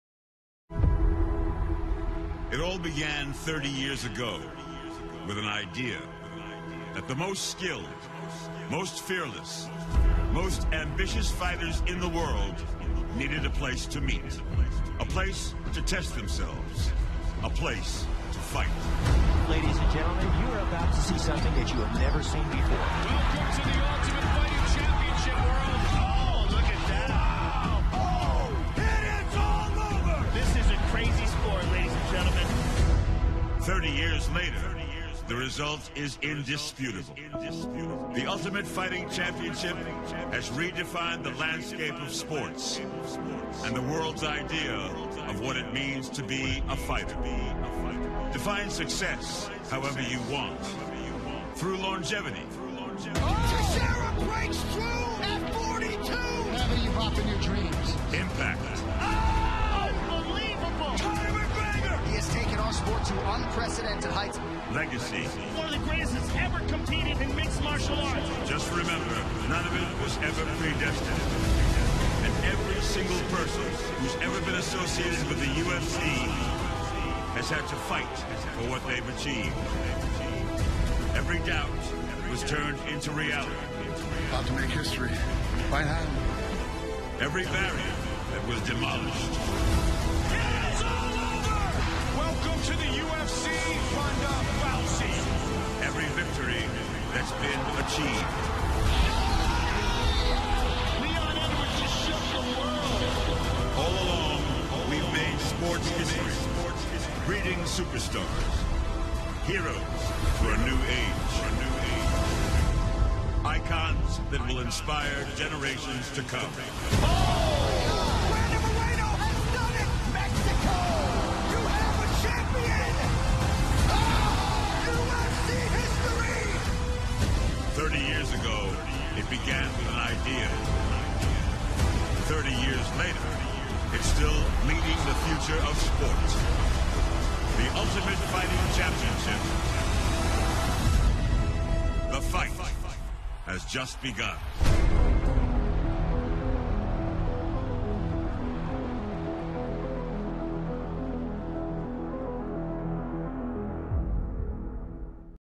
UFC - 30 years
Za tři desetiletí se UFC rozrostlo z fenoménu na bezkonkurenční sílu a vytvořilo některé z nejpamátnějších okamžiků a rozpoznatelných hvězd v historii sportu a ostatní nechalo v patách.